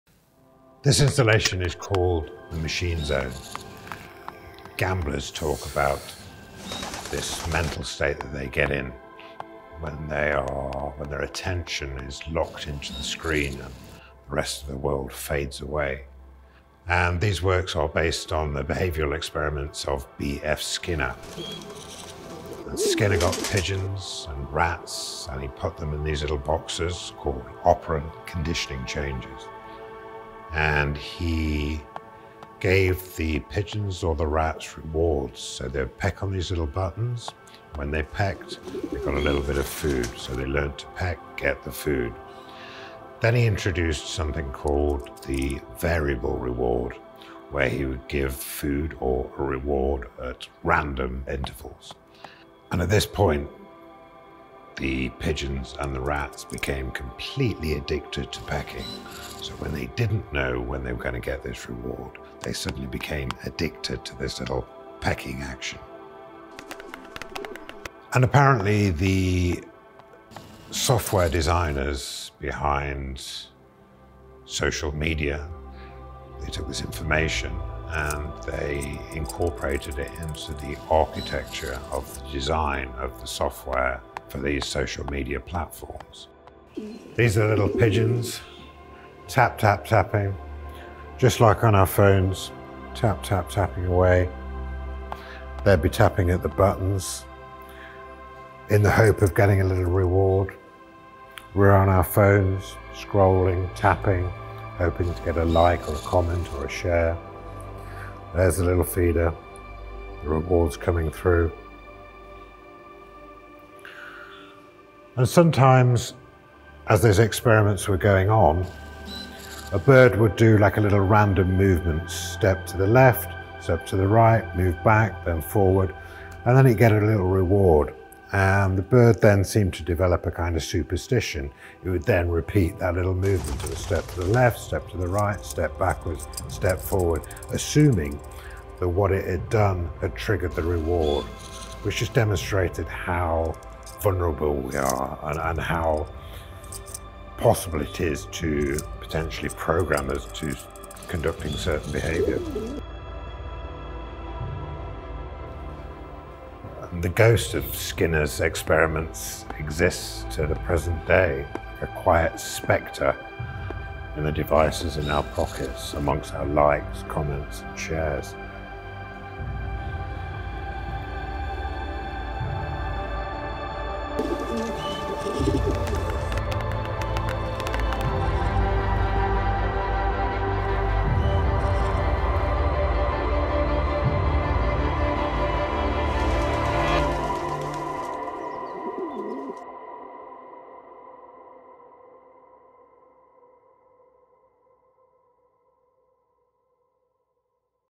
Mat Collishaw: The Machine Zone
Artist Mat Collishaw introduces The Machine Zone, a new work for our exhibition 24/7: A Wake-Up Call For Our Non-Stop World. The work explores the idea of random reward which underpins the algorithms driving social media interactions.